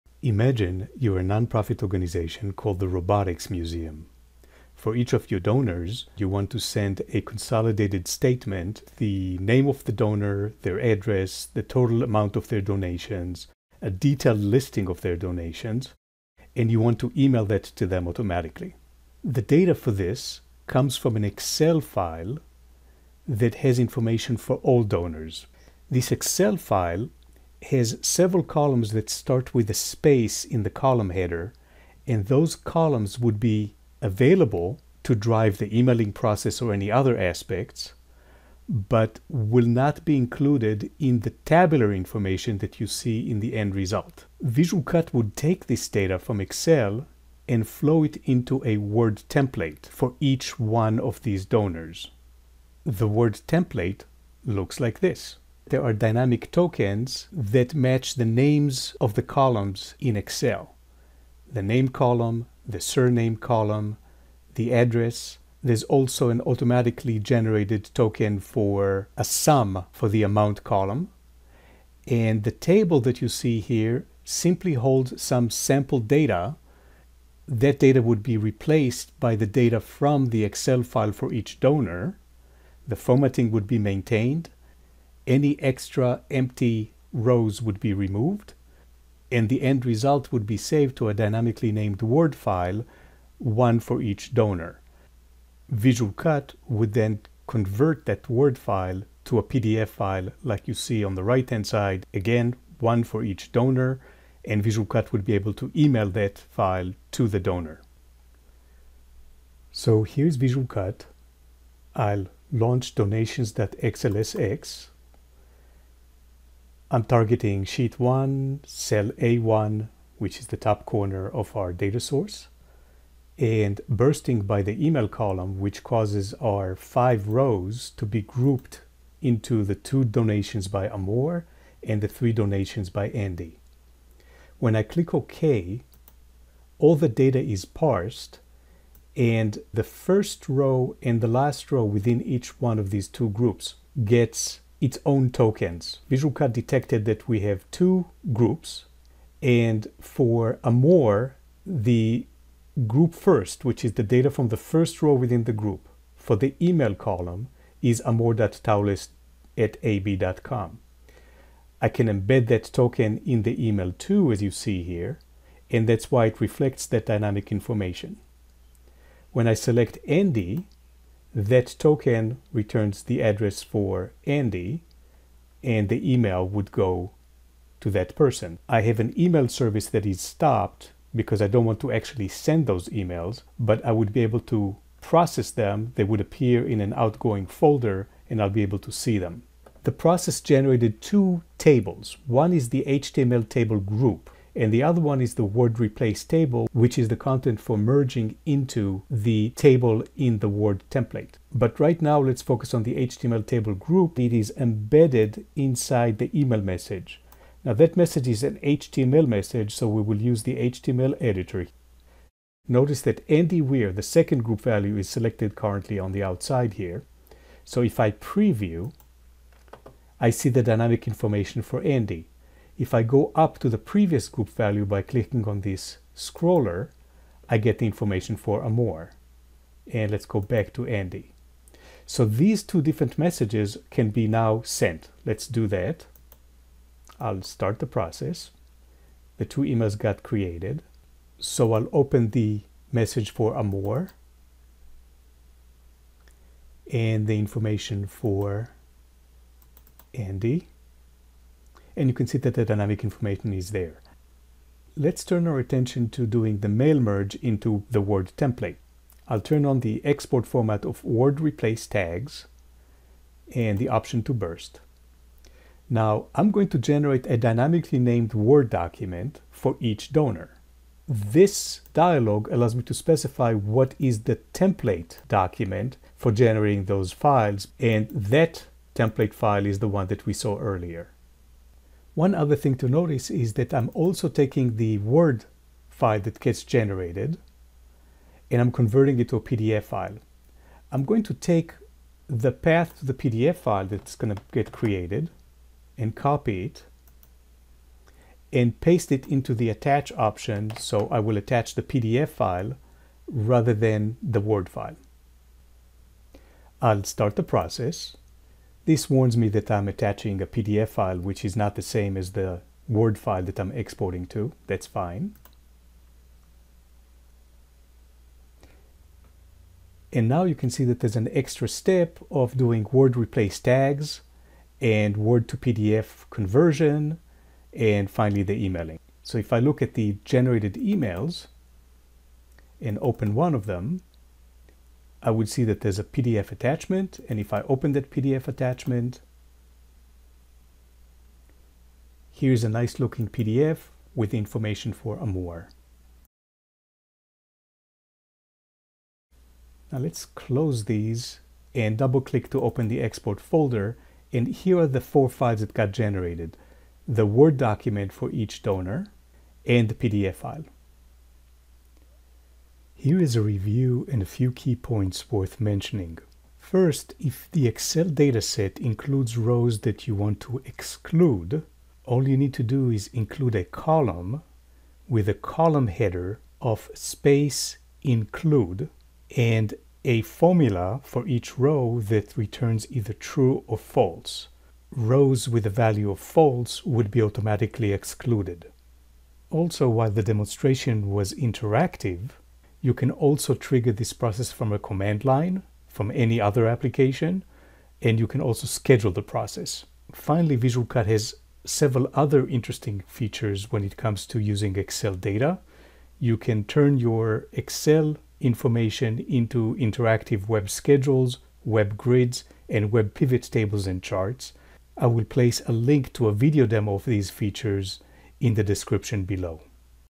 Mail Merge Excel Data Into Word Templates and Email the Results
Demonstrates using Visual CUT to mail merge excel data into Word template containing a table and individual tokens, converting to pdf, and email to dynamic email destinations.

Link to related video demo of converting excel data to Web Schedules, Web Grids, and Web Pivot Tables/Charts

Link to related video demo demonstrating email bursting Excel data and excluding rows based on formulas in an ' Include' column